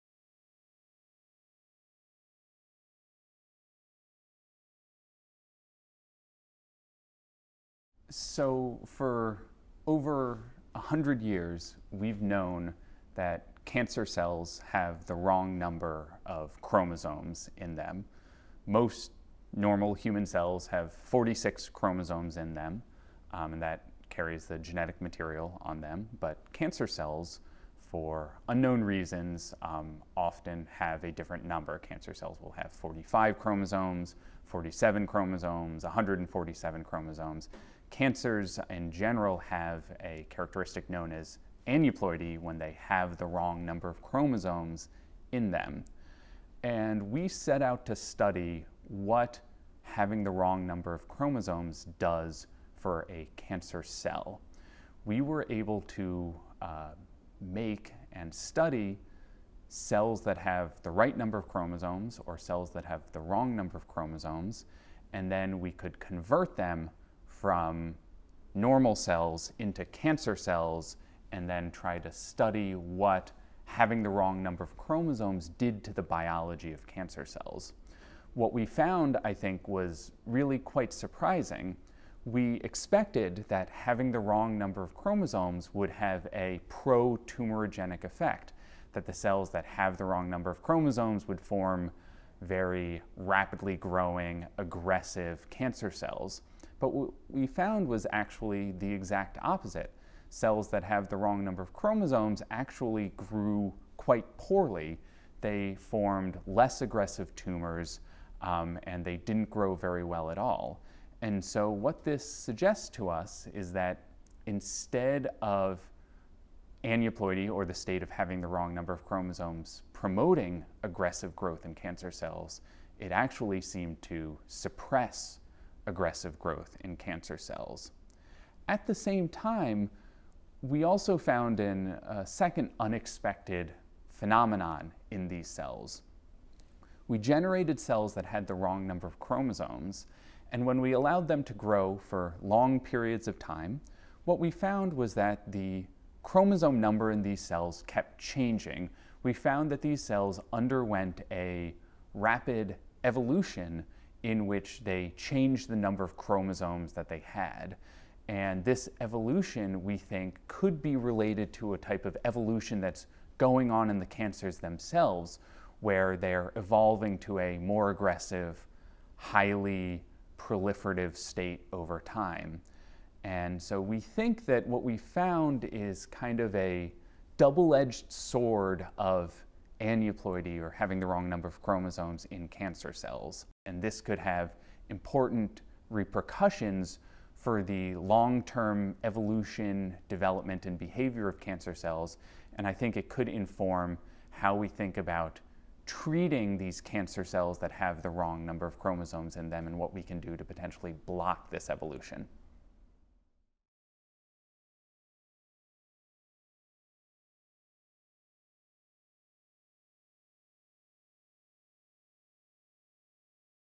Relationship between incorrect chromosome number and cancer is reassessed
Pre-malignant aneuploid cells grew more slowly and formed smaller tumors than comparable cells with normal chromosome number.

Today in the journal Cancer Cell, Cold Spring Harbor Laboratory (CSHL) Fellow Jason Sheltzer, Ph.D., and colleagues at CSHL and MIT report surprising results of experiments intended to explore the consequences of having too many or too few chromosomes, a phenomenon that biologists call aneuploidy (AN-you-ploid-ee).